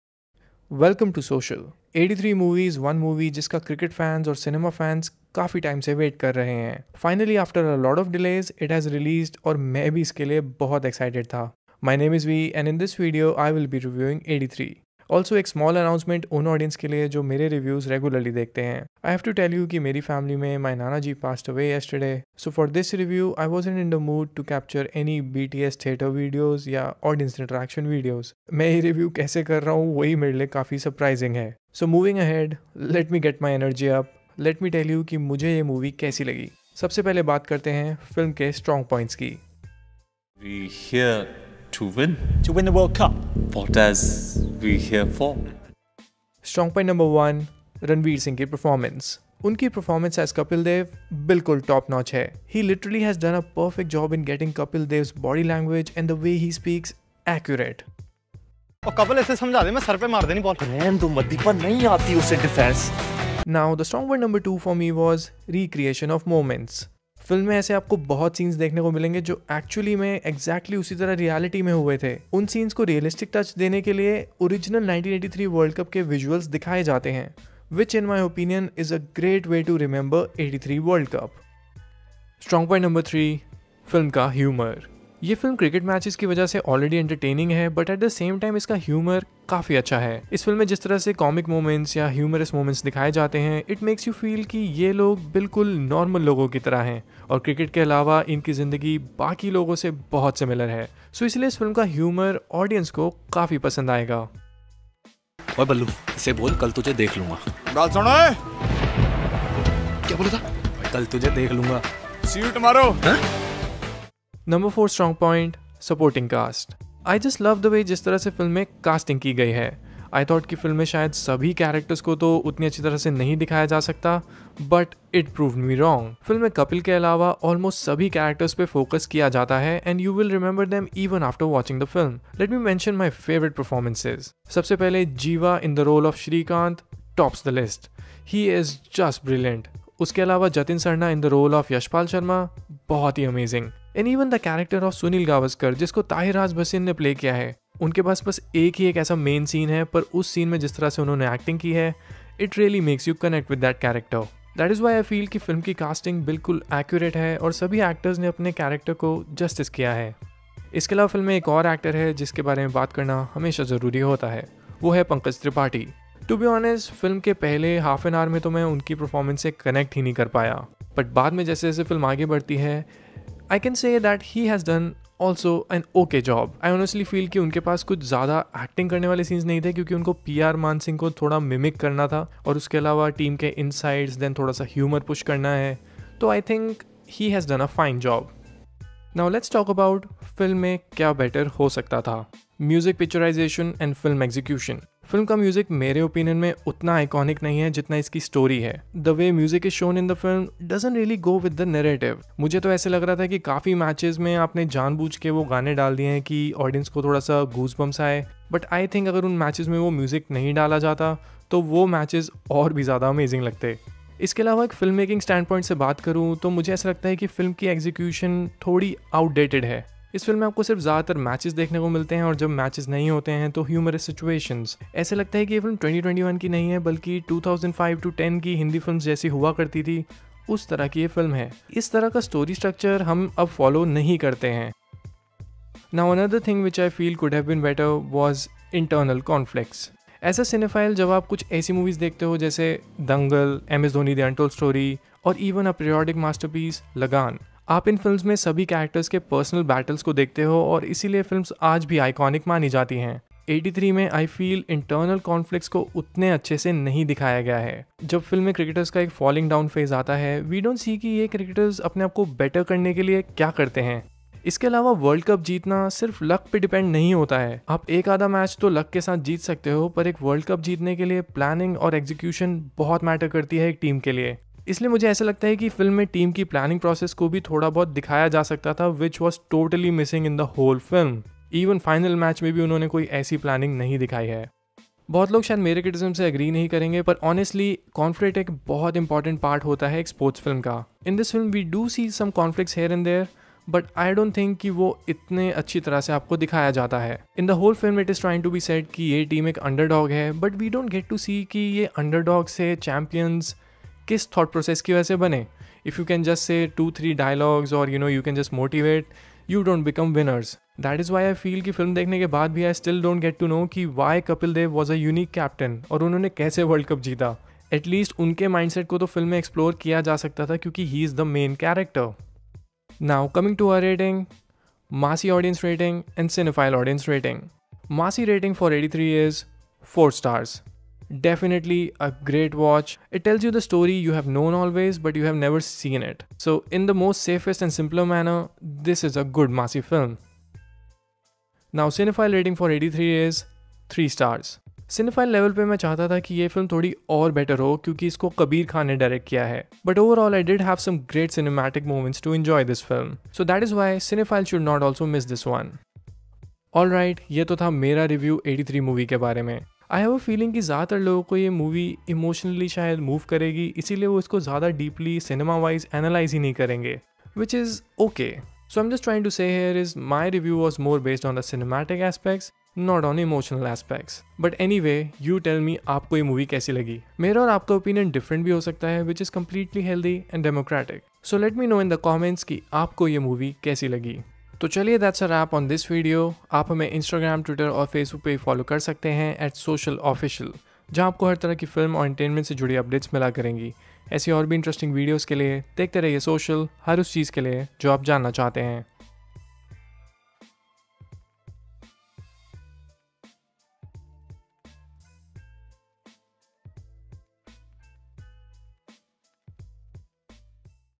83 Movie Honest Cinematic Review | Ranveer Singh
This time 5ocial brings you the review of 83, the movie based on Indian cricket teams journey of 1983 World cup. Starring Ranveer Singh, Deepika Padukone, Pankaj Tripathi, Tahir Raj Bhasin, Jiiva, Saqib Saleem and many more. Directed by Kabir Khan.

Review by Vi

Other Titles for Better Search ►
83 Movie Hindi Review
83 Movie Scenes
83 Movie Real Story Kapil Dev
83 Movie Ranveer Singh